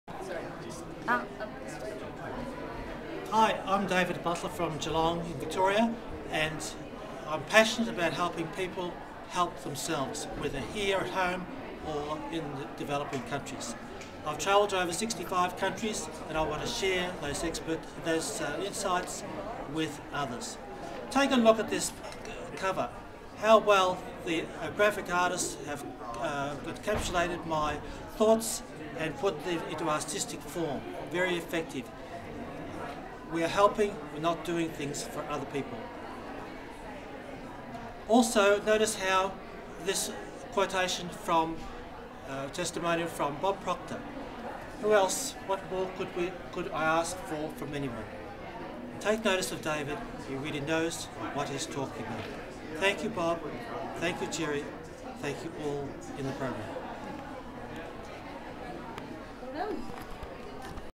The Author Mentorship Program with Gerry Robert
- Testimonial from Super Dave about publishing a book. Gerry Robert is a successful book publisher shows people how to use a book as a marketing tool. His website is freeauthortoolkit.com. AUTHORS WANTED. He used to own a book publishing company with Bob Proctor from The Secret.